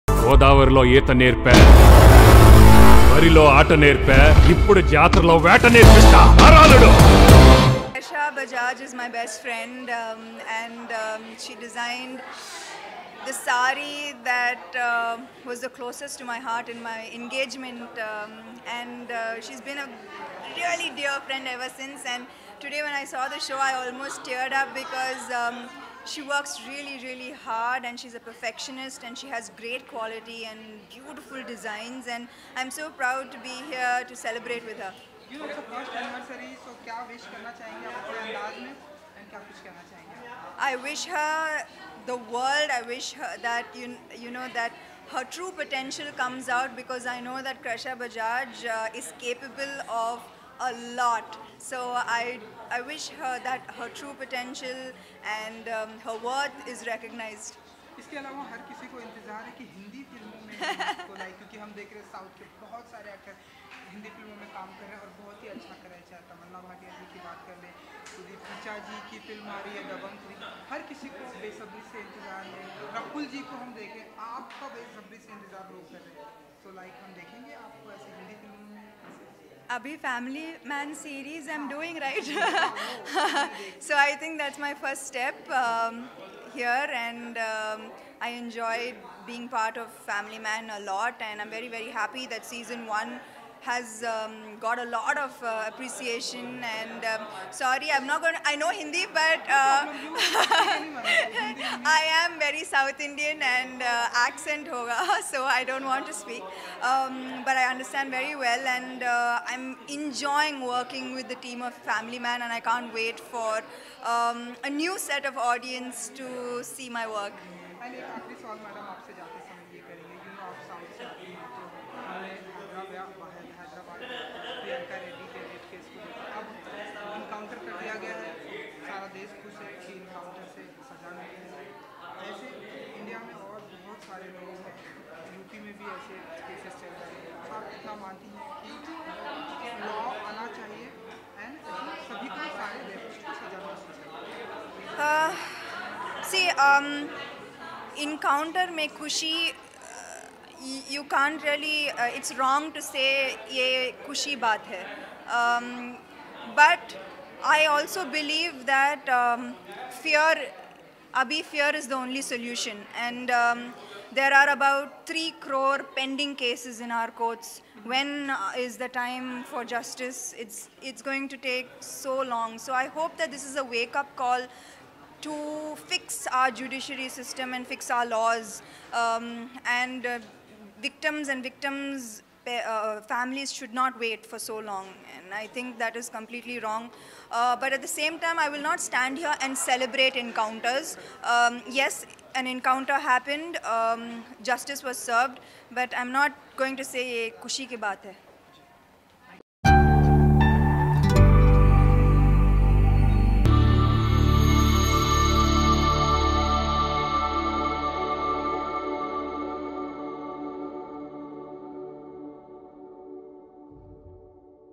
Samantha Akkineni Reveals about her Bollywood Debut in Family Man Web Series - Filmyfocus.com
Samantha Akkineni Reveals about her Bollywood Debut in Family Man Web Series.